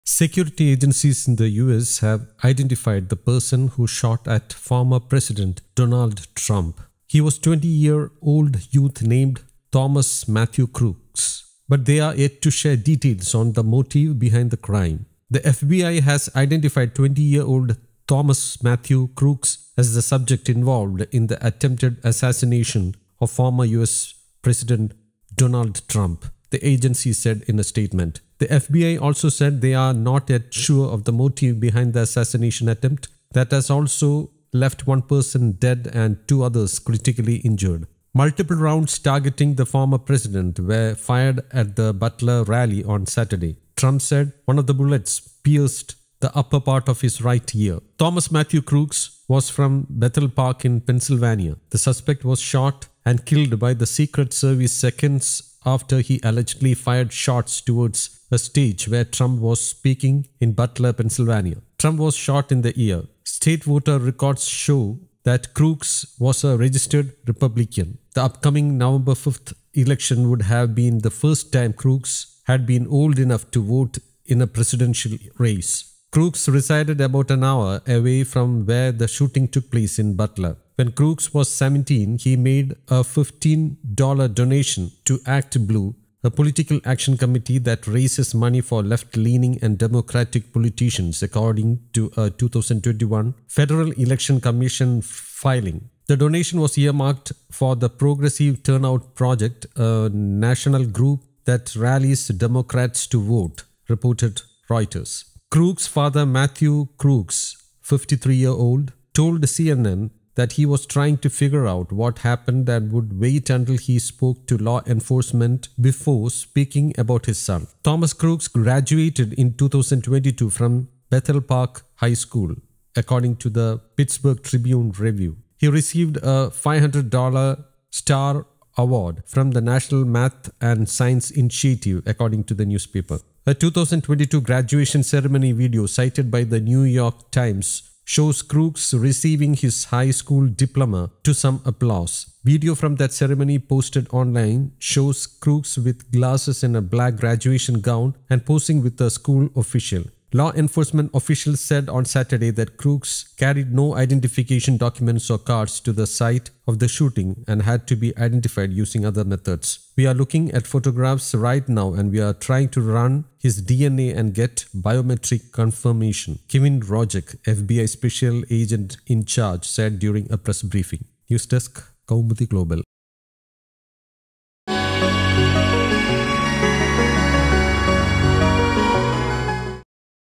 What was Trump Rally Shooter’s motive?
Security agencies in the US have identified the person who shot at former President Donald Trump. He was 20-year-old youth named Thomas Matthew Crooks. But they are yet to share details on the motive behind the crime. The FBI has identified 20-year-old Thomas Matthew Crooks as the "subject involved" in the attempted assassination of former President Donald Trump, the agency said in a statement. The FBI also said they are not yet sure of the motive behind the assassination attempt, that has also left one person dead and two others critically injured.